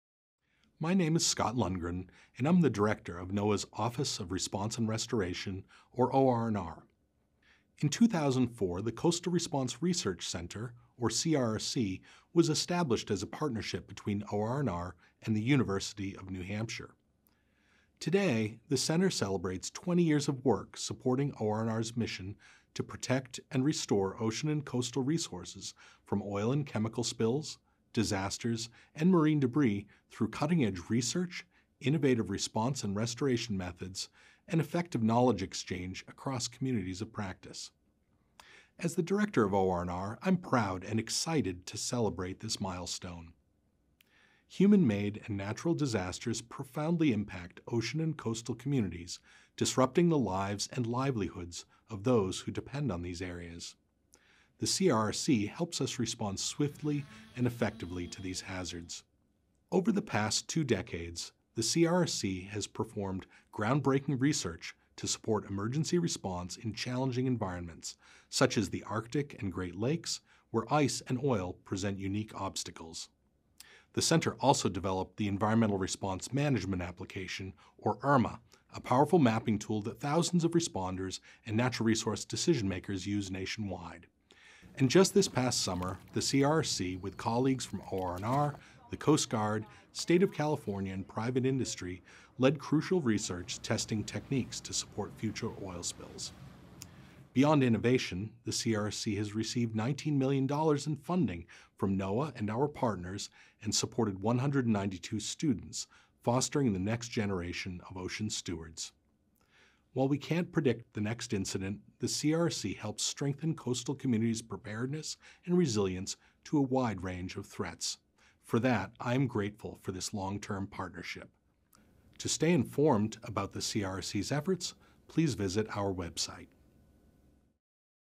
Coastal Response Research Center - Celebrating 20 Years of Partnership
In this video message Scott Lundgren, director, NOAA’s Office of Response and Restoration, reflects on the importance and impacts of the 20 year partnership between NOAA and the University of New Hampshire.